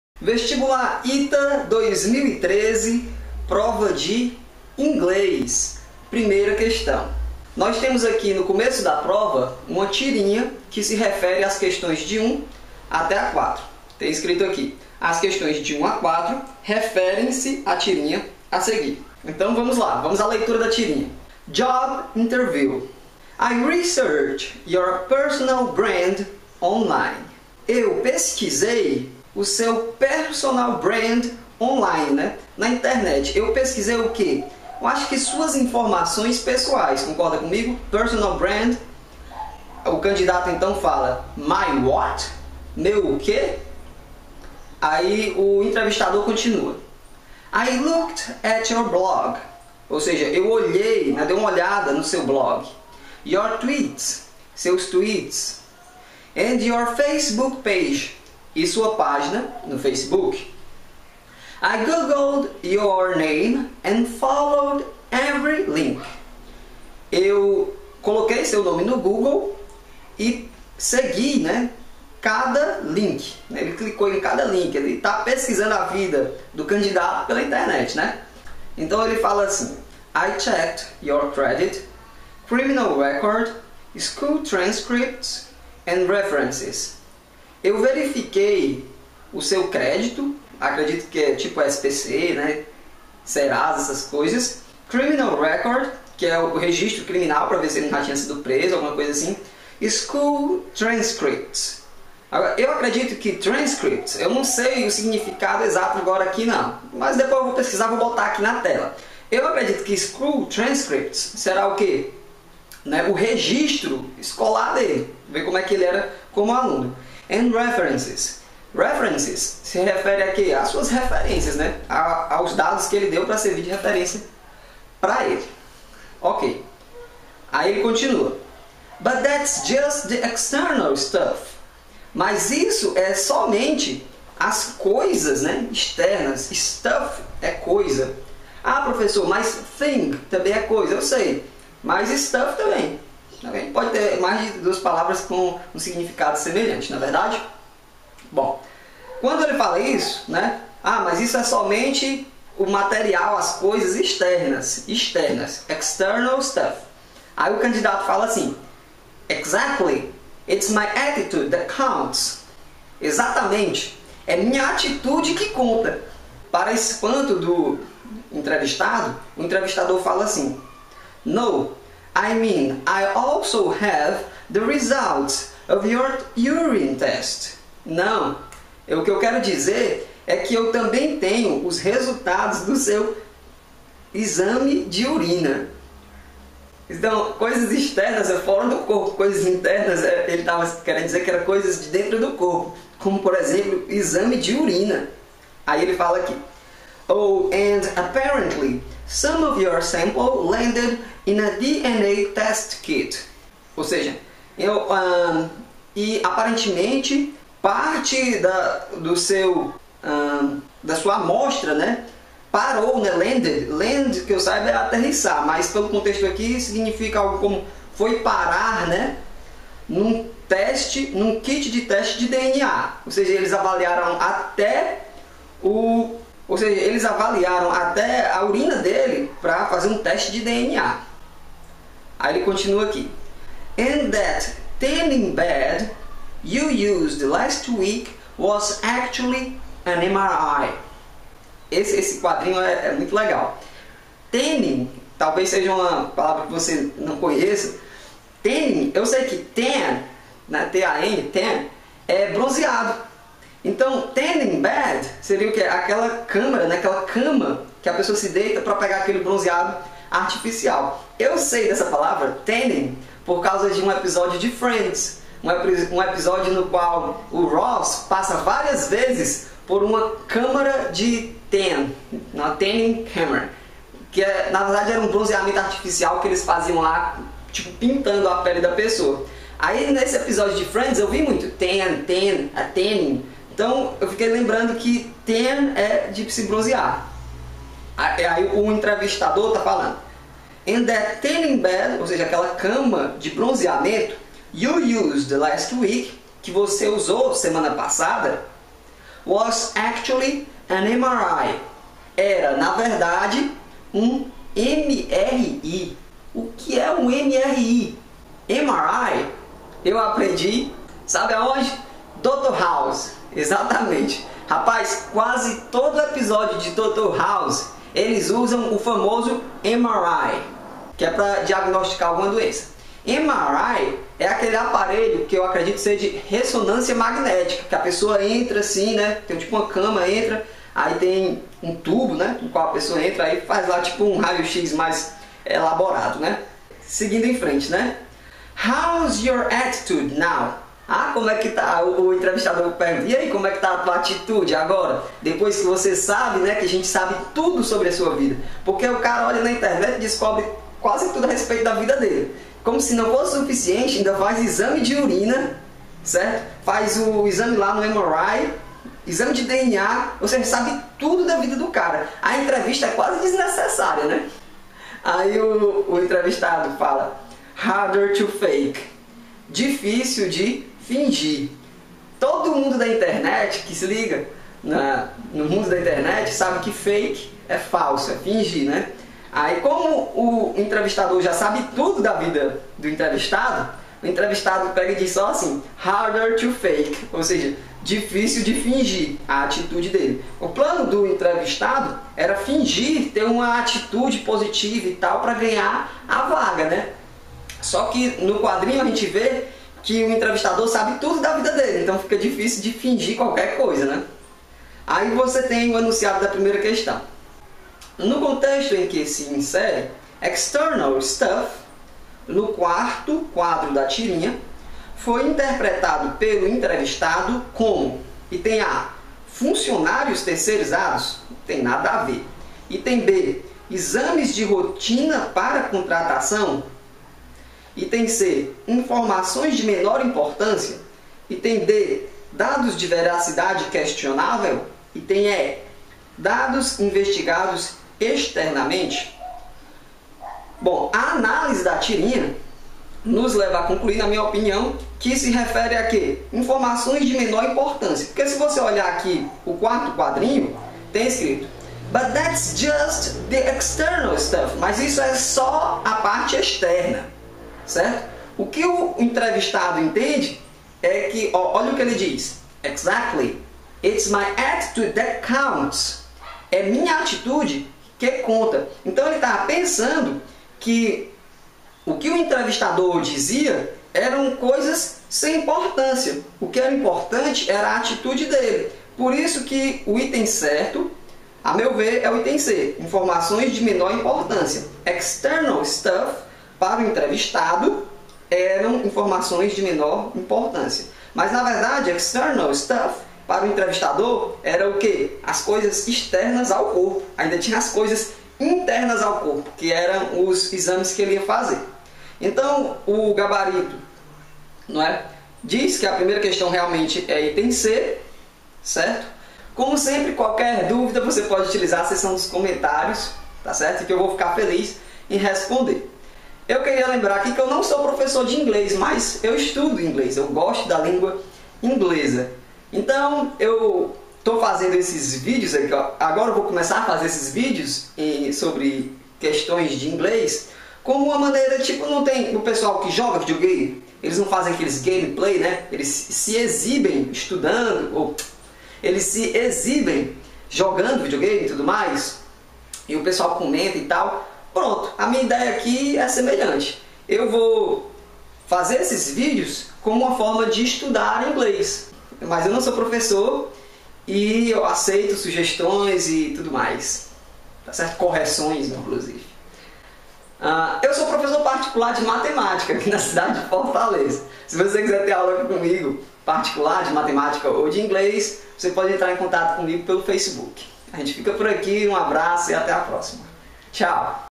Job Interview (ITA, 2013, INGLÊS, Questão 1)
Questão 1. No contexto em que se insere, "external stuff", no quarto quadro da tirinha, foi
interpretado, pelo entrevistado, como
A ( ) funcionários terceirizados.
B ( ) exames de rotina para contratação.
C ( ) informações de menor importância.
D ( ) dados de veracidade questionável.
E ( ) dados investigados externamente.